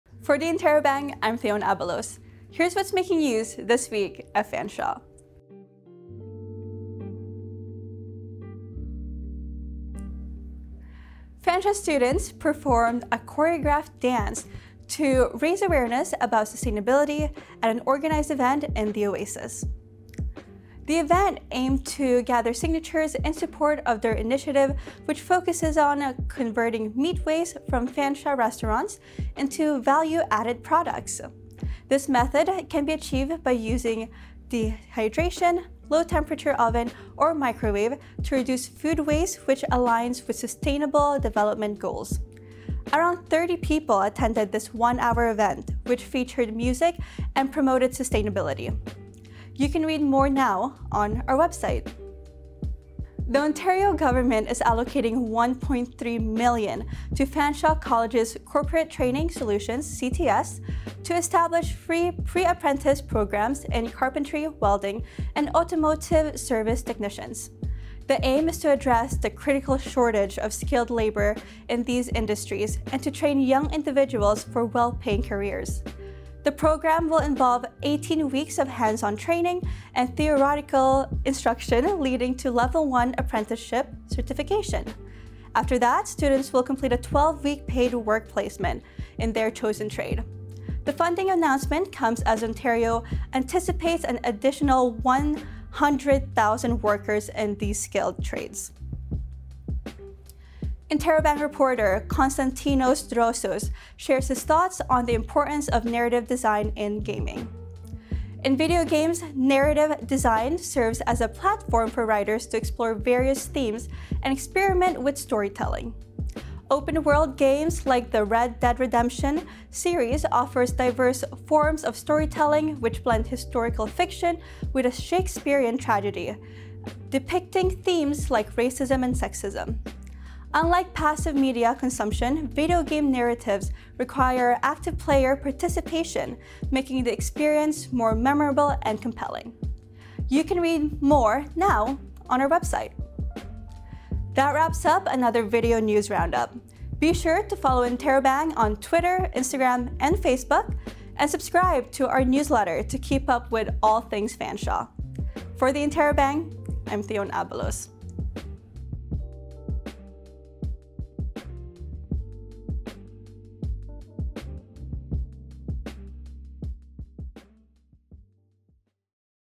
Video News Roundup (August 10, 2023)
Interrobang video reporter Theone Abalos catches you up on the latest stories making news this week at Fanshawe. The Ontario government is investing $1.3 million towards a free skilled trades program at Fanshawe, students dance for a cause, and reporter Konstantinos Drossos shares his thoughts on narrative design in video games.